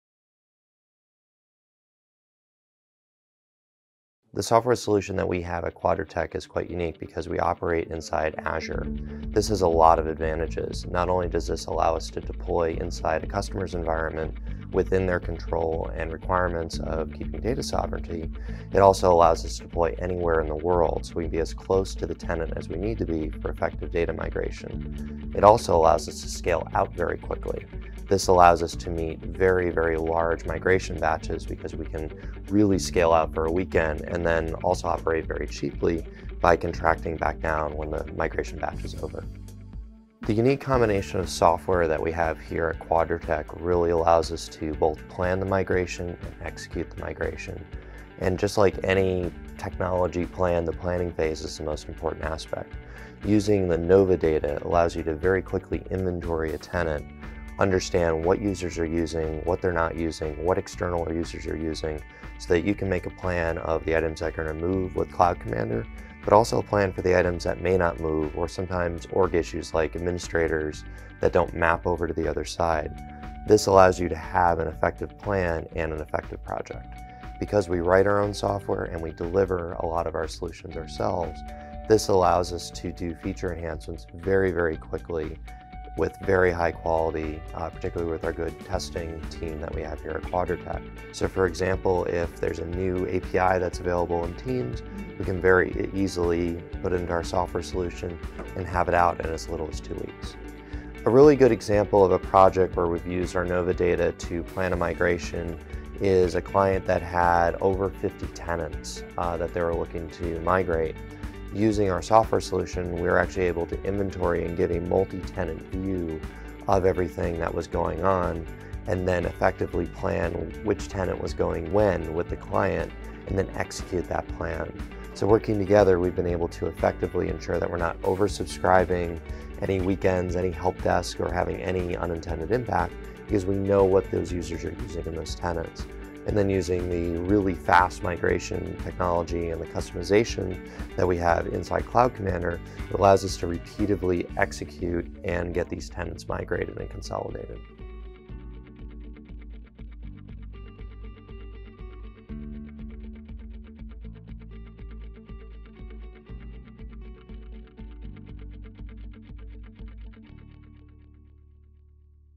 Office 365 to Office 365 Migration Tool - Cloud Commander
Quadrotech's powerful Office 365 to Office 365 Migration Tool, Cloud Commander, migrates core workloads between tenants.